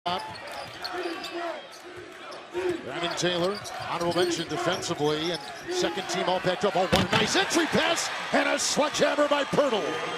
Stephen F. Austin vs. Utah: Jakob Poeltl dunk
Utah runs the pick and roll to perfection and is rewarded with the big Jakob Poeltl one-handed slam.